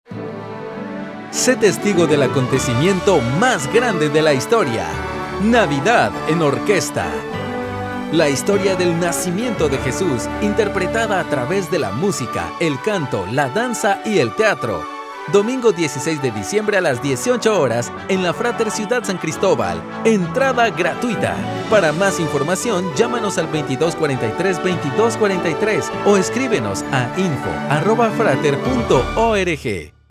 Navidad en Orquesta 2018
¡No te pierdas de Navidad en Orquesta! La historia del nacimiento de Jesús contada a través de la música, el canto, la danza y el teatro.

Recuerda la historia del nacimiento de nuestro Señor Jesucristo y reflexiona acerca del impacto que este acontecimiento tuvo en la humanidad entera.

📆Domingo 16 de diciembre
🕔18:00 hrs.
📍La Fráter Ciudad San Cristóbal.

La entrada es gratuita, contamos con amplio parqueo.